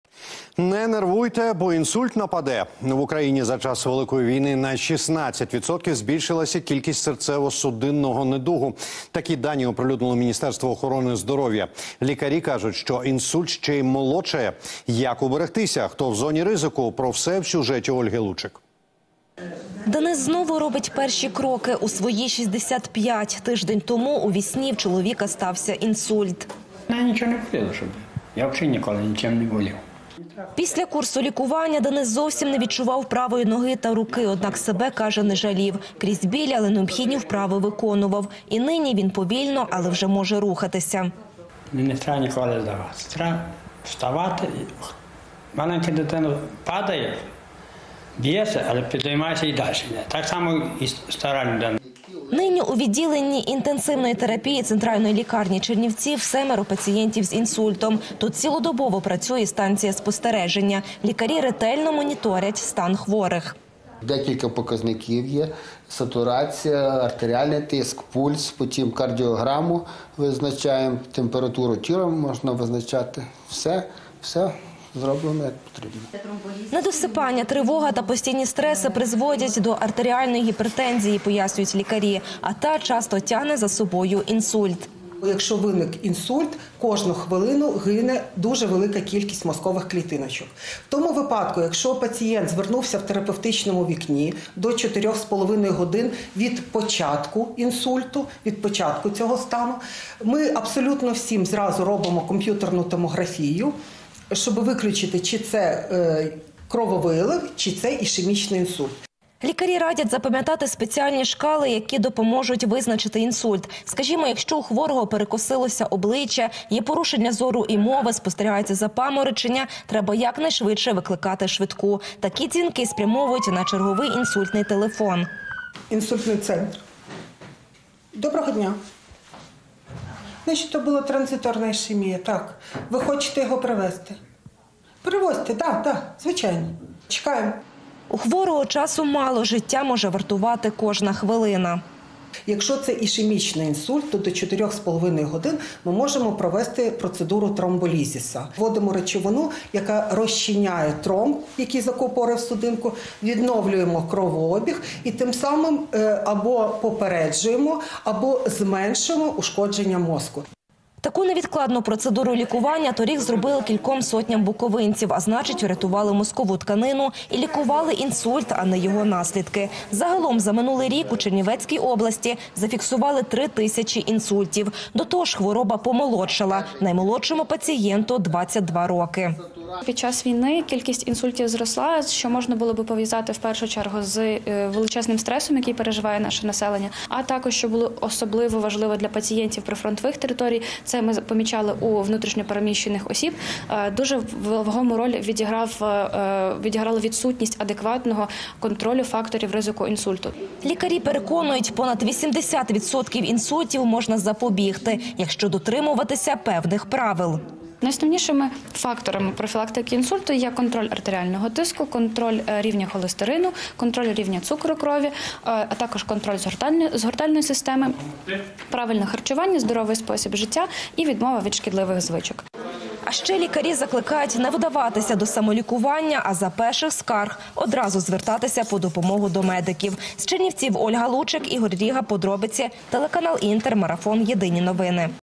Інсульт молодшає: як вберегтися
Не нервуйте! Бо інсульт підстерігає! В Україні за час великої війни на 16% збільшилася кількість серцево-судинного недугу. Такі дані оприлюднило Міністерство охорони здоров'я. Лікарі ж кажуть, що інсульт ще й помолодшав. Як вберегтися? Хто в зоні ризику? Про все в сюжеті Ольги Лучек.